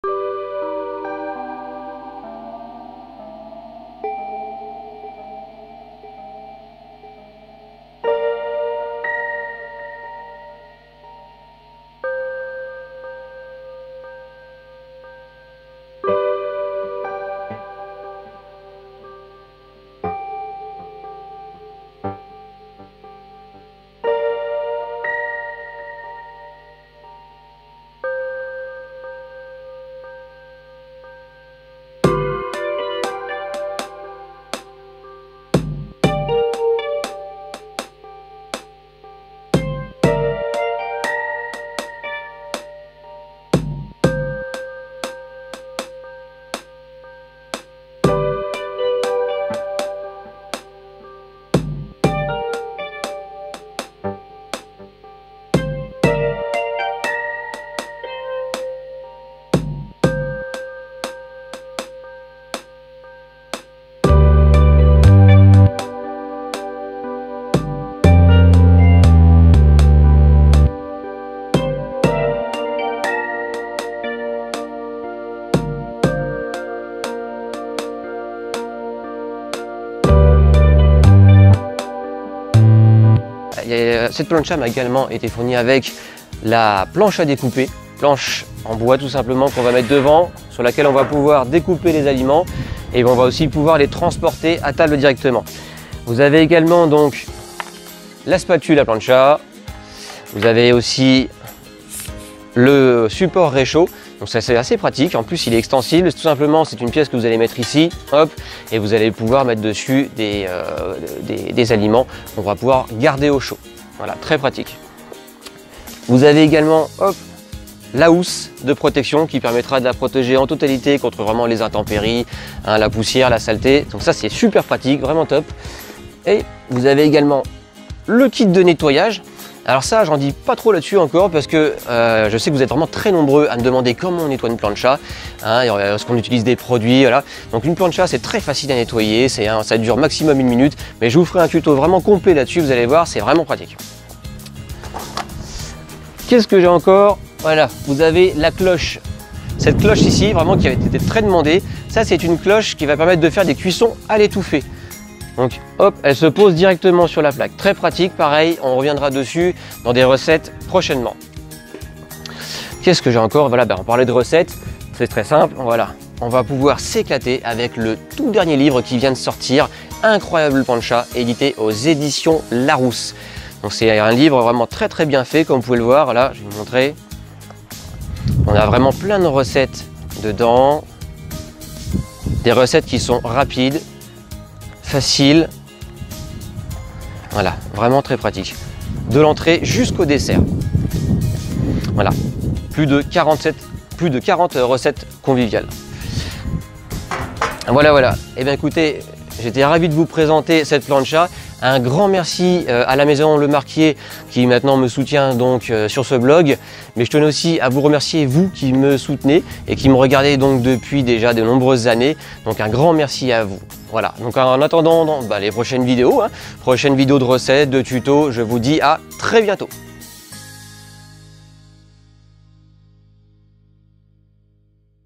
LE TOUT PREMIER PARTENAIRE DE MON BLOG, C’EST UNE MAISON FRANÇAISE: LES PLANCHAS LE MARQUIER
Je suis très heureux de vous présenter le tout premier partenaire de mon Blog, c'est une maison Française: les Planchas Le Marquier!

Découvrez la Barbecue Weber Master Touch E5770